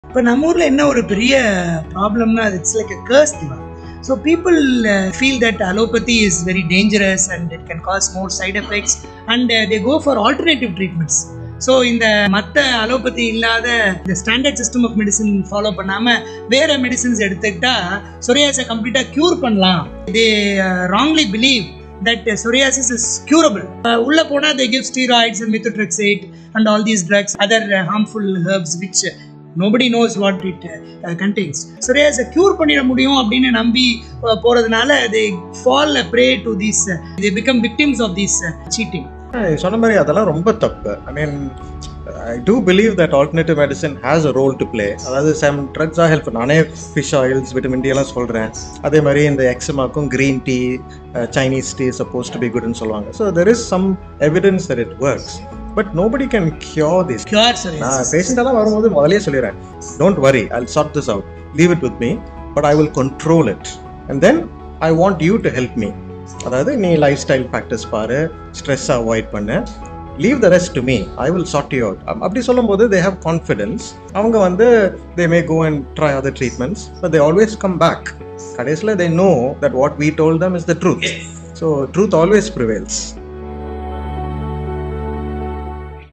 Don’t Believe the Myths: How Doctors Treat Psoriasis Today
Episode - 6
Part 4
Think allopathy can’t cure psoriasis? That’s a myth. 💊 Two expert dermatologists discussed how modern treatments—from topical solutions to biologics—can effectively control symptoms and improve life quality. Don’t fall for misinformation. Trust proven science and consult your skin expert today. 🧴
Follow Dr.S Murugusundram
Founder & Medical Director,
Chennai Skin Foundation & Yesudian Research Institute
[PsoriasisMyth, SkinFacts, DermatologyExperts, AllopathyWorks, PsoriasisCare, SkinHealth]